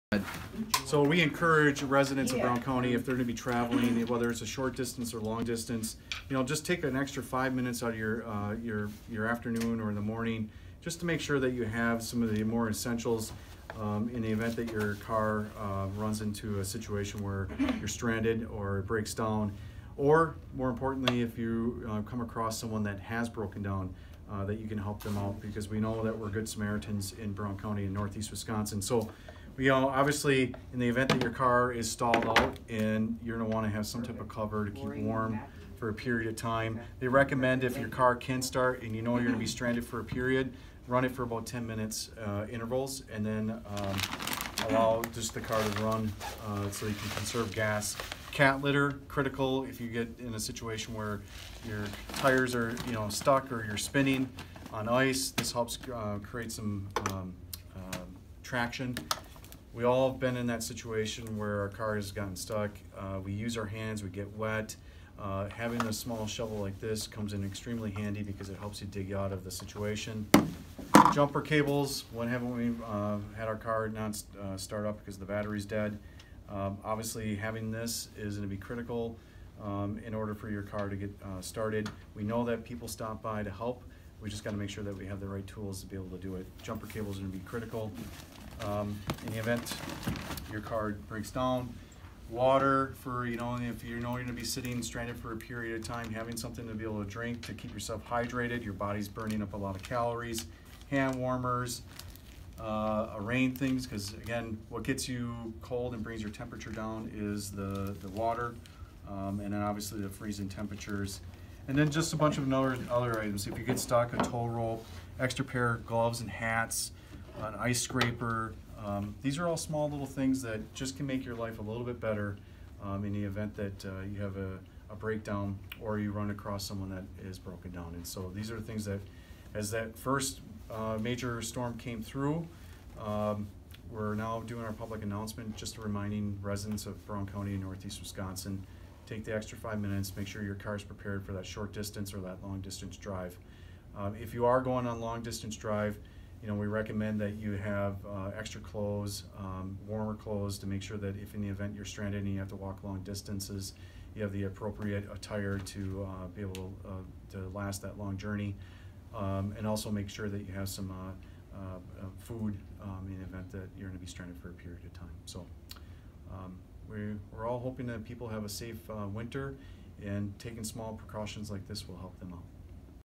Winter Driving Preparation
Brown County Executive Troy Streckenbach shows what items should be in an Emergency Kit for your car.